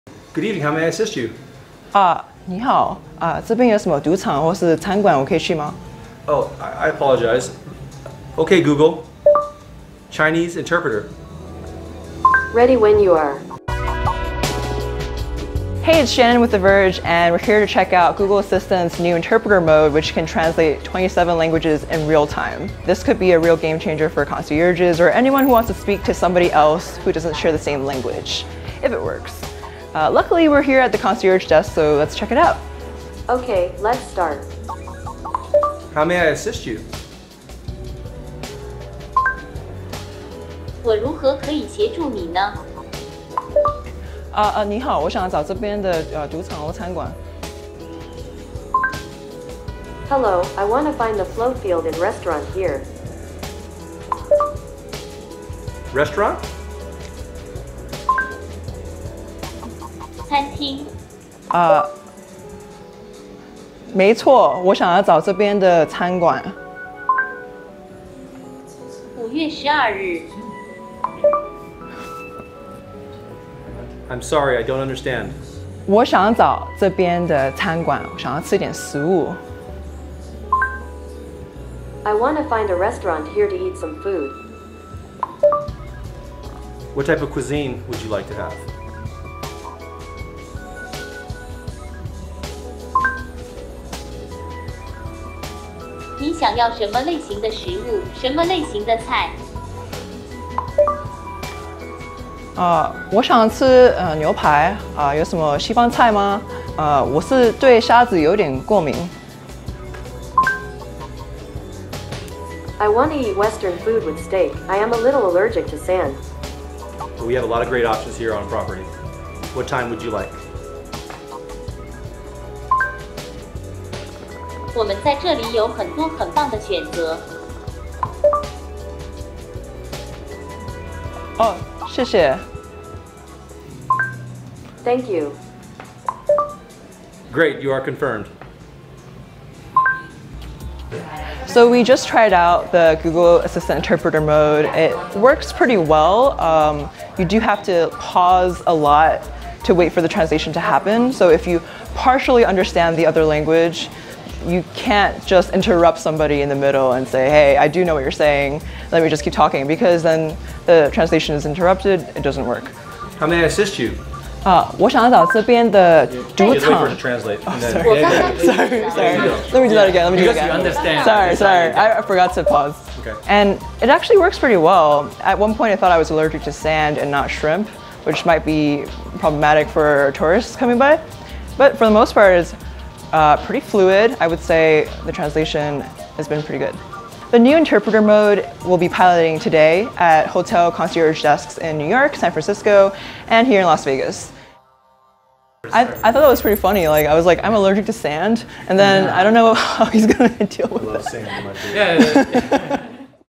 Google Assistant’s interpreter mode translates 27 languages
Google Assistant will soon be able to act as your real-life translator in 27 different languages. Google announced today that the voice assistant is getting a new “interpreter mode” that can translate in real time so you can hold conversations with someone who doesn’t share the same tongue. It works, but it’s not magic.

CORRECTION: Google Assistant's Chinese interpreter only includes Mandarin, not Cantonese.